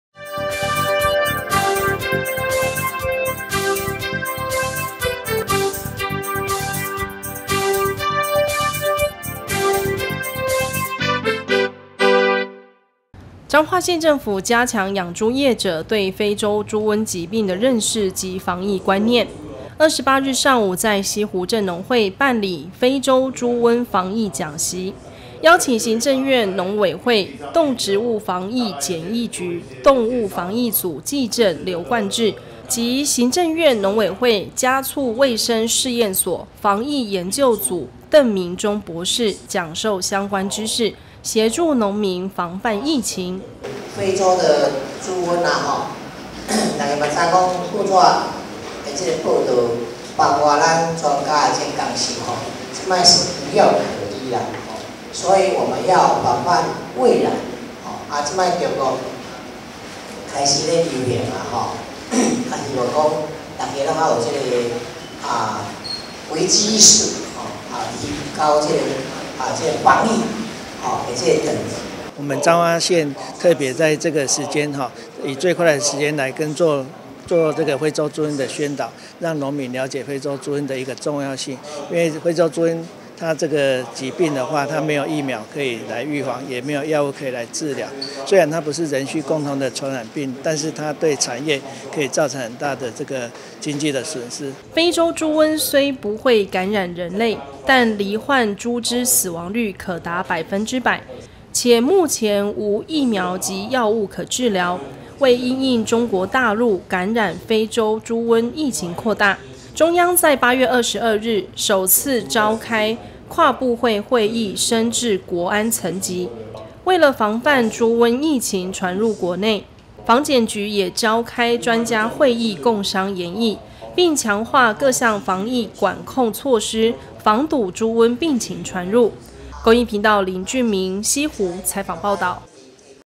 107-08-28 防範非洲豬瘟疫情入侵 防疫講習協助養豬業者加強防範
彰化縣政府加強養豬業者對非洲豬瘟疾病的認識及防疫觀念，28日上午在溪湖鎮農會辦理非洲豬瘟防疫講習，邀請行政院農委會動植物防疫檢疫局動物防疫組技正劉冠志，及行政院農委會家畜衛生試驗所防疫研究組鄧明中博士講授相關知識，協助農民防範疫情。
非洲豬瘟雖不會感染人類，但罹病豬隻死亡率可達100%，且目前無疫苗及藥物可治療。為因應中國大陸感染非洲豬瘟疫情擴大，中央在8月22日首次召開跨部會會議升至國安層級，為了防範豬瘟疫情傳入國內，防檢局也召開專家會議共商研議，並強化各項防疫管控措施，防堵豬瘟病情傳入。